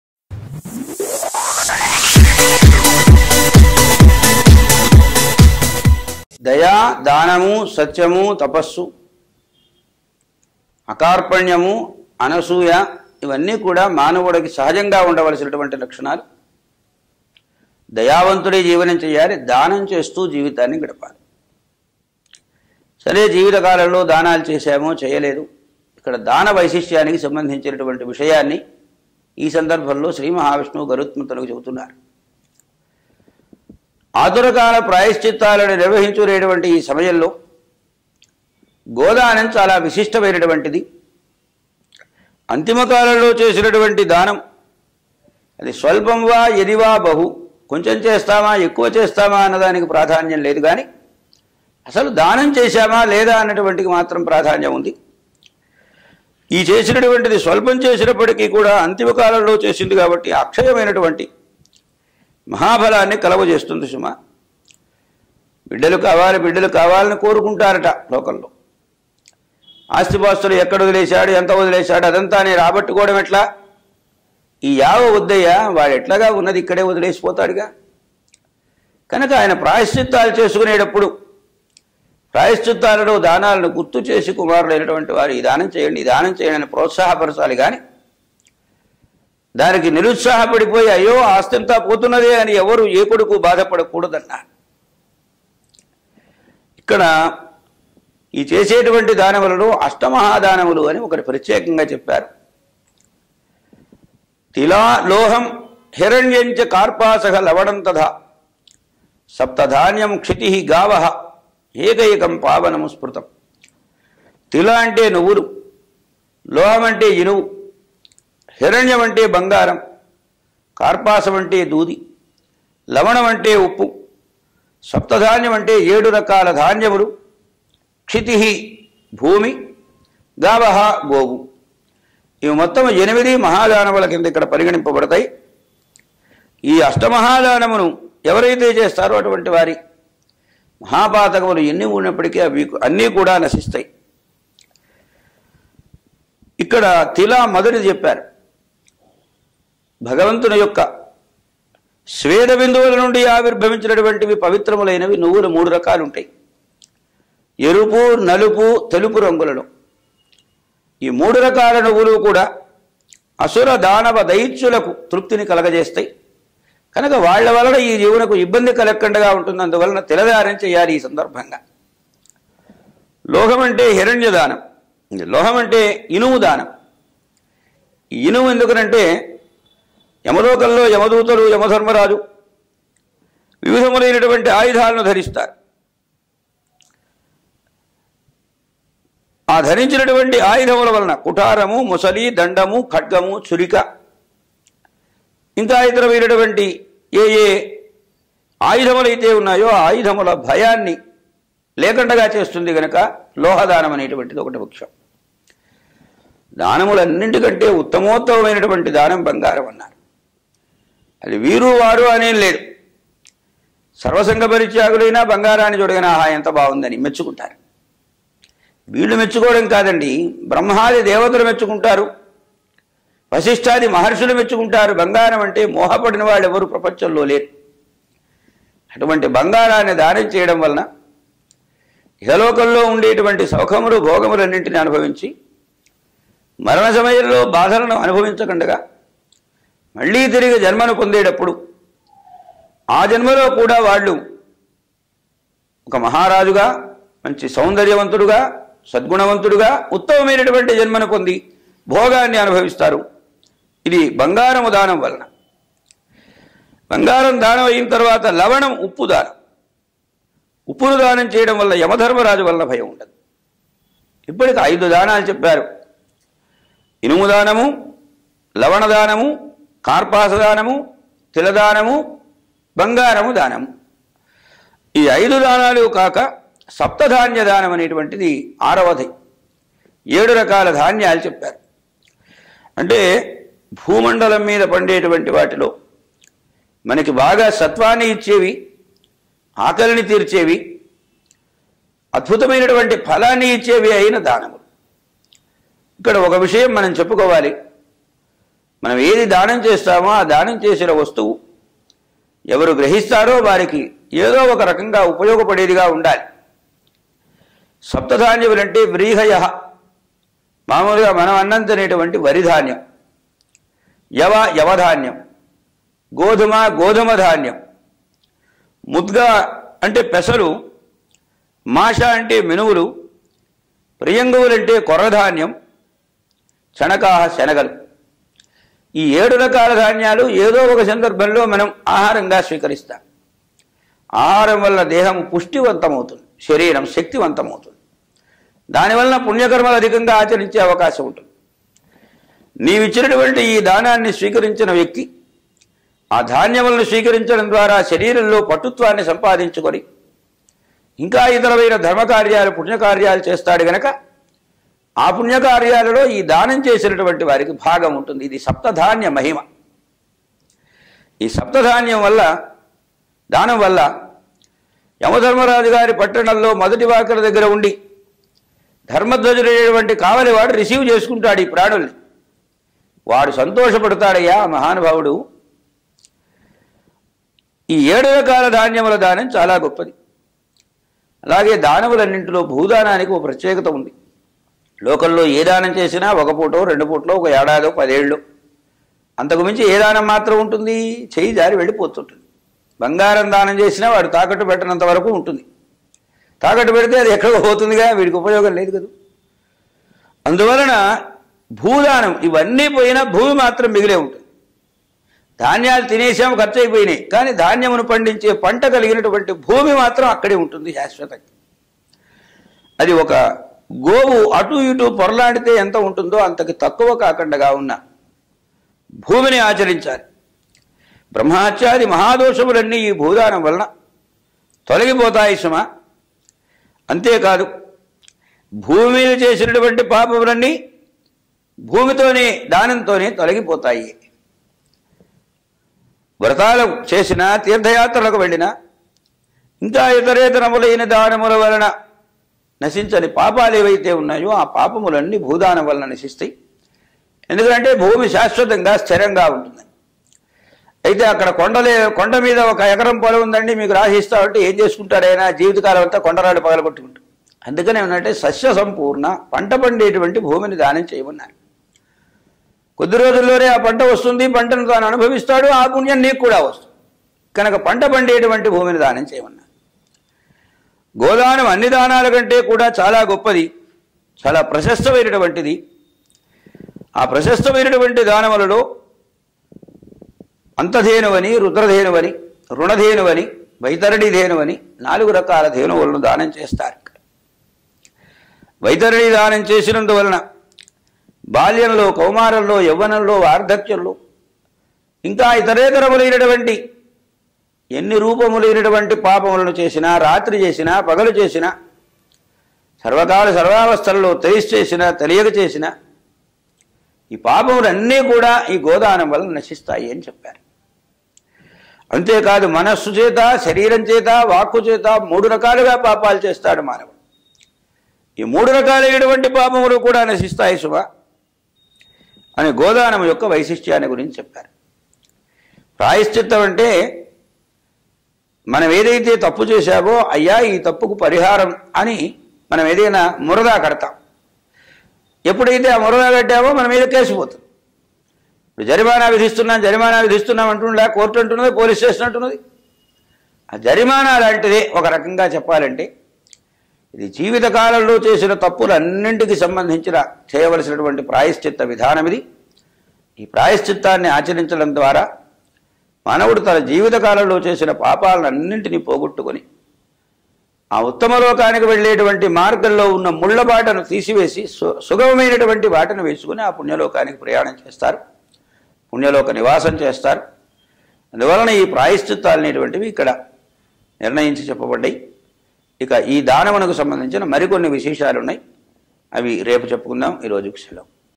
Importance Of Donation In Human Life - Garuda Puranam In Telugu | Somasi Balagangadhara Sharma
Garuda Puranam By Somasi Balagangadhara Sharma